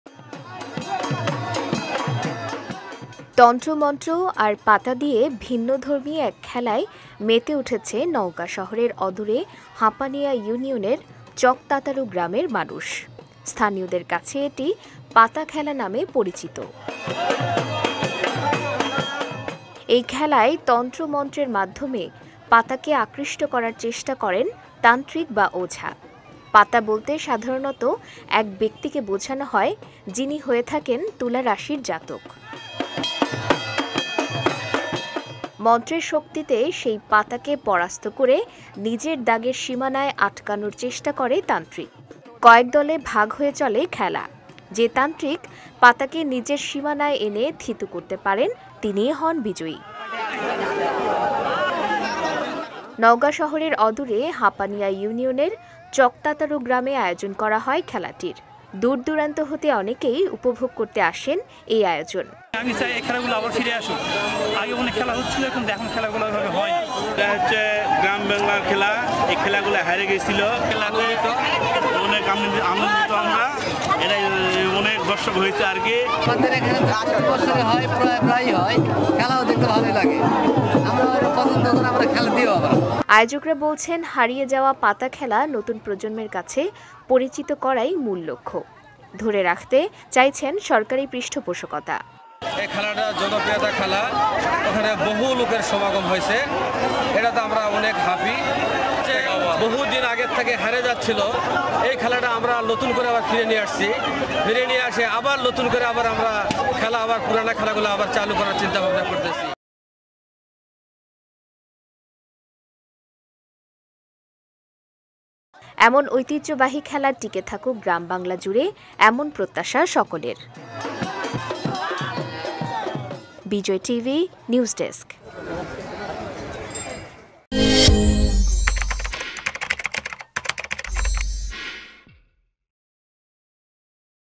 হারিয়ে যাচ্ছে গ্রাম বাংলার ঐতিহ্যবাহী পাতা খেলা | Bijoy TV
তান্ত্রিক পাতা খেলা; কালের বিবর্তনে হারাতে বসেছে ঐতিহ্যবাহী এই গ্রামীণ খেলাটি। তাই নতুন প্রজন্মের কাছে পরিচিত করতে নওগাঁ শহরের অদূরে হাঁপানিয়া ইউনিয়নের চকতাতারু গ্রামে আয়োজন করা হয় খেলাটির। খেলা দেখতে ভিড় জমায় সব বয়সী মানুষ।
তন্ত্রমন্ত্র আর পাতা দিয়ে ভিন্নধর্মী এক খেলায় মেতে উঠেছে নওগাঁ শহরের অদূরে হাঁপানিয়া ইউনিয়নের চকতাতারু গ্রামের মানুষ। স্থানীয়দের কাছে এটি পাতা খেলা নামে পরিচিত।
এই খেলায় তন্ত্র-মন্ত্রের মাধ্যমে পাতাকে আকৃষ্ট করার চেষ্টা করেন তান্ত্রিক বা ওঝা। পাতা বলতে সাধারণত এক ব্যক্তিকে বোঝানো হয় যিনি হয়ে থাকেন তুলা রাশির জাতক।